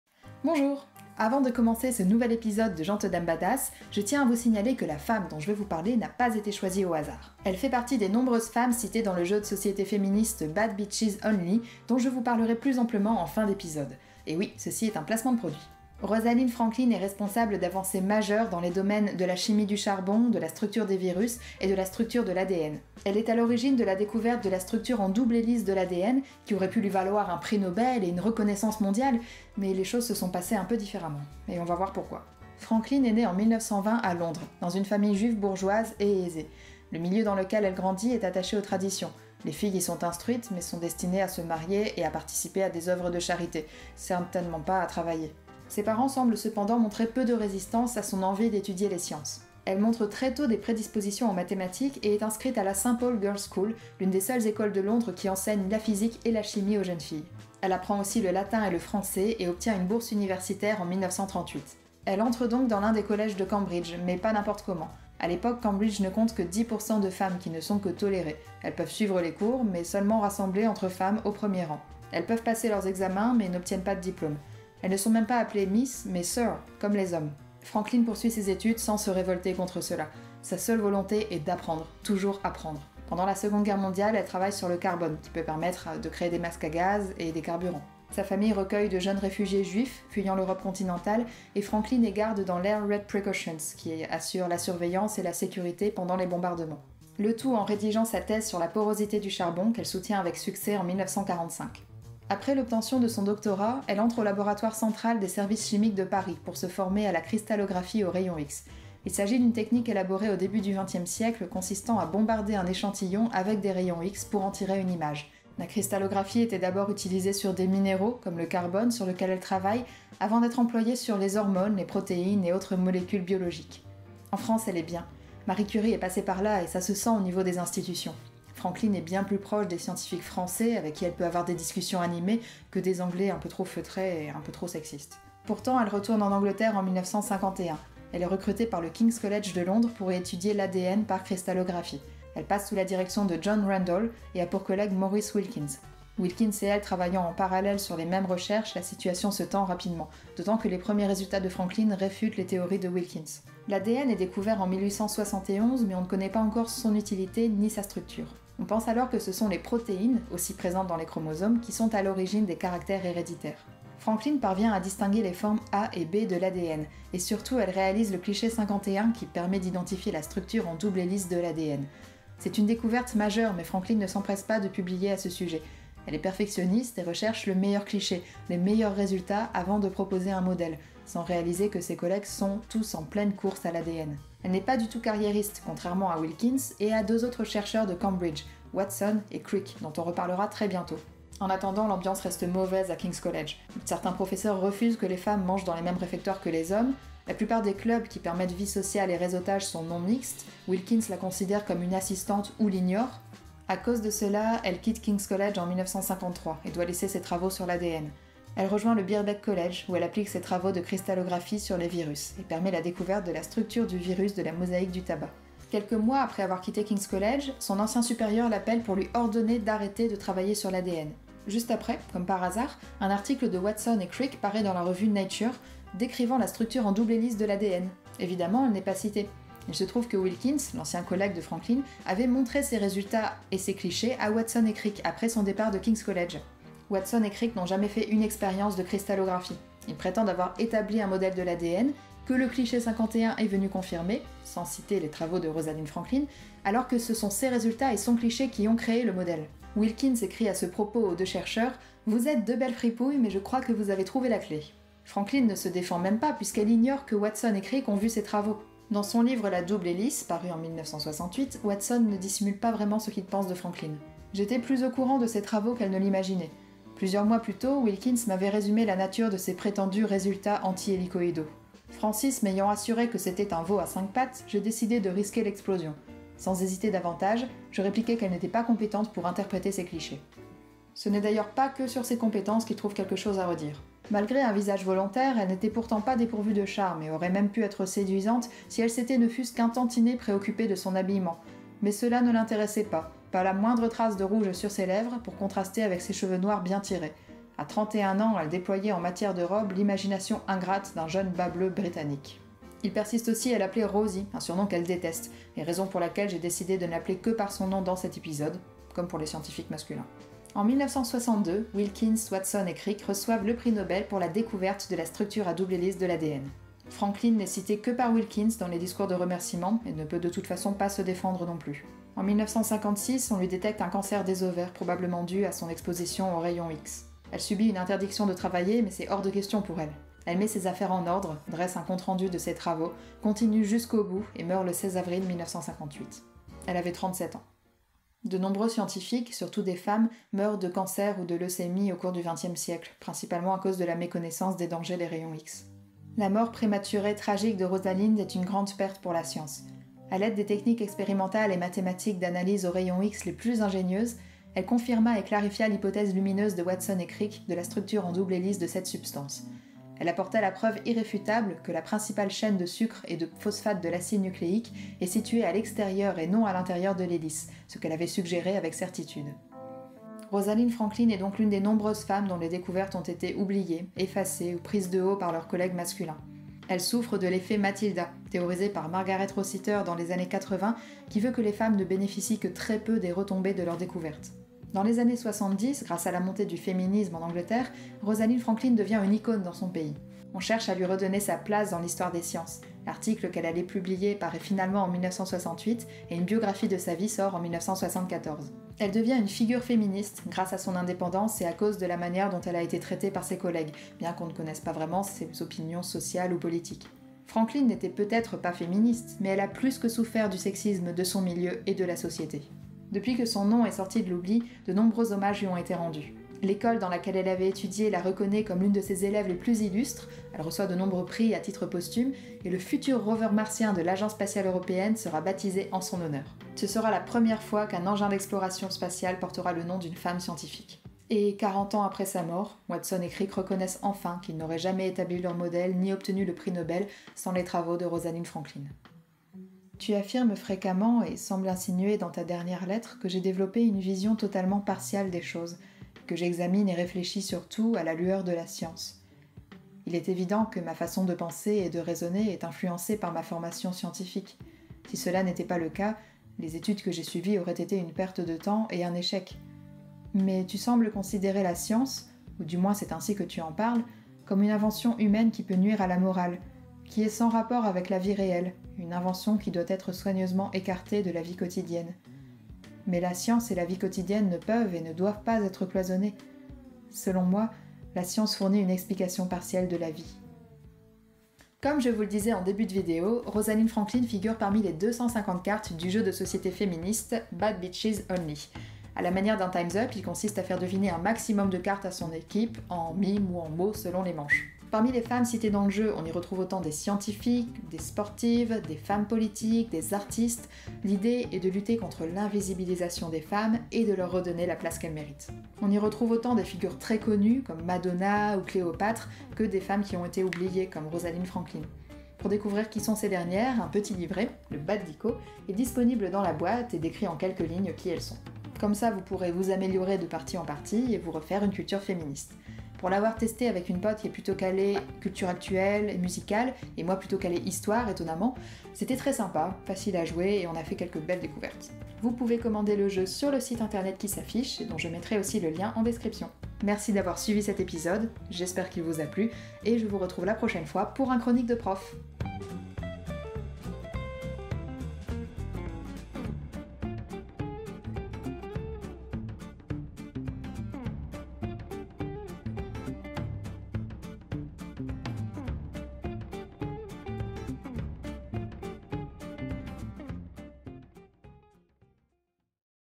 Rosalind Franklin - Gentes Dames Badass #16
Rosalind Franklin : une scientifique, une découverte... Et peu de reconnaissance à la clé.